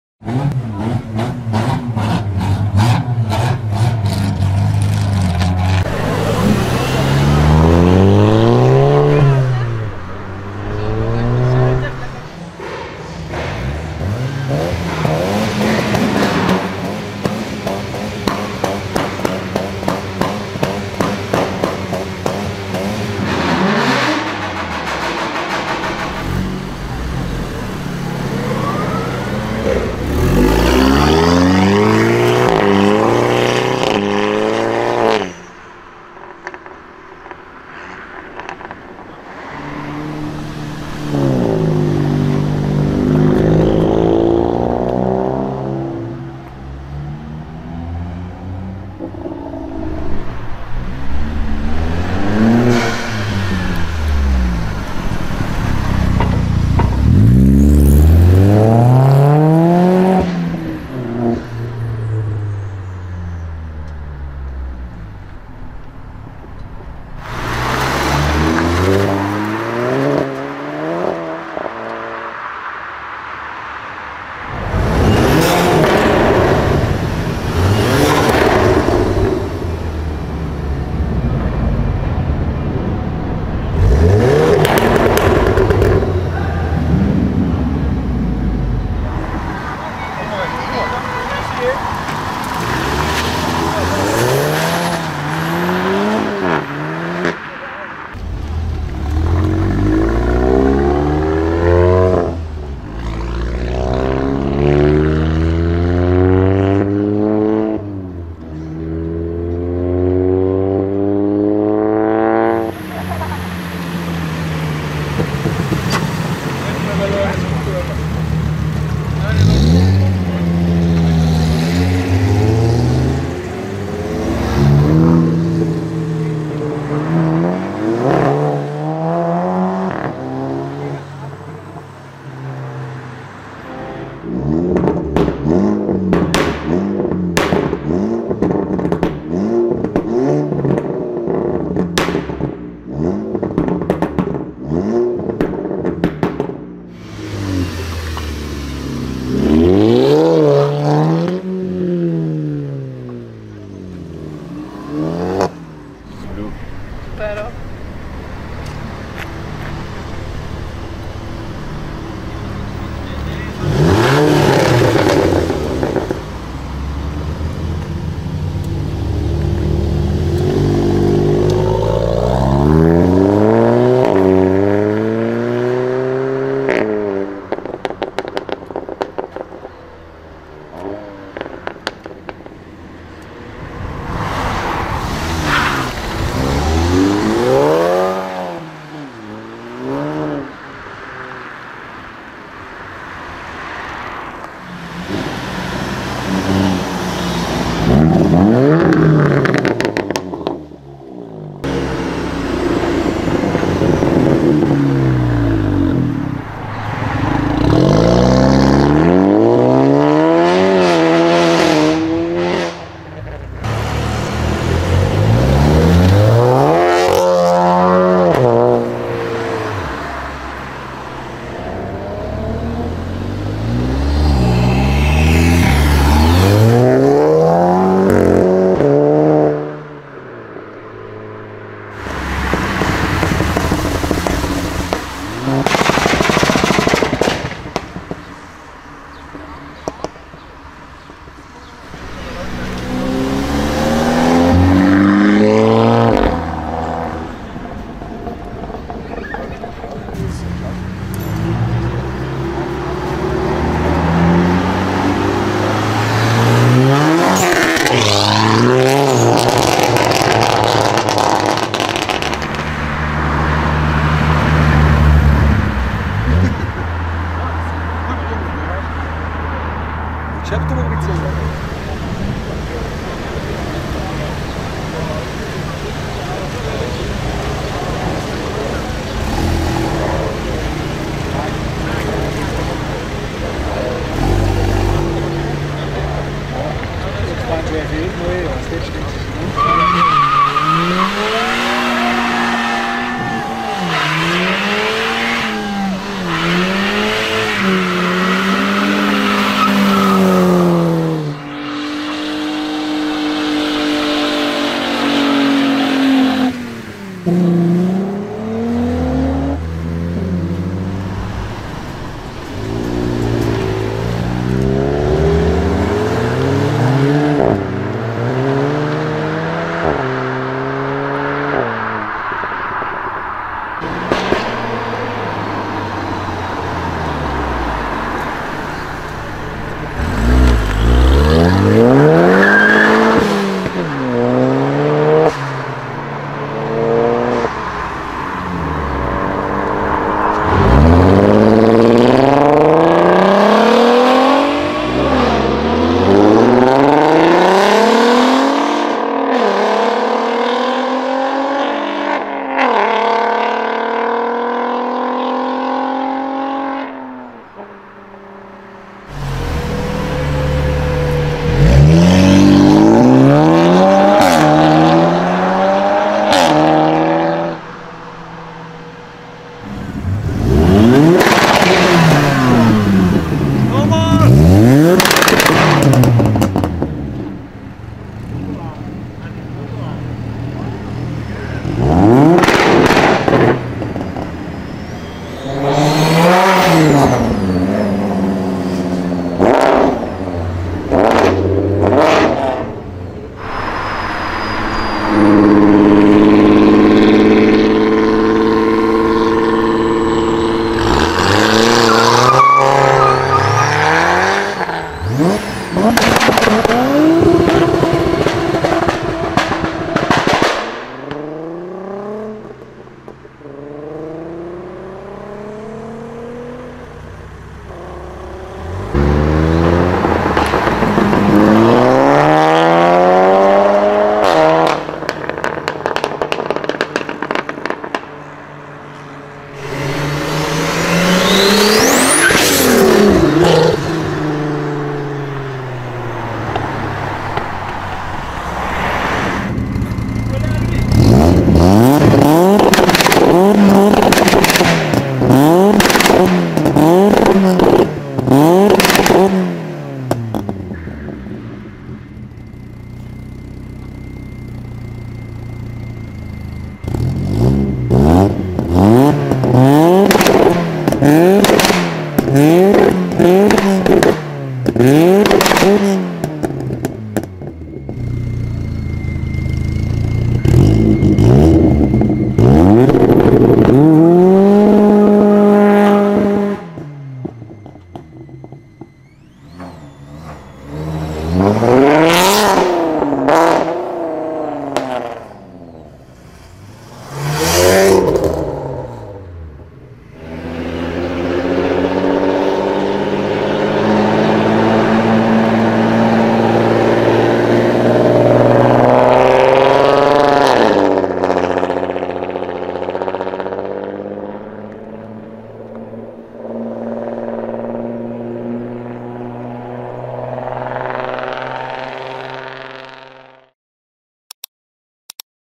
2019 BEST OF VOLKSWAGEN GOLF SOUND! - Exhaust Revs, Loud Accelerations & Burnouts(HD)!
-The YSR-

Video about the best of Volkswagen Golf Sound, Accelerations & Burnouts of 2019!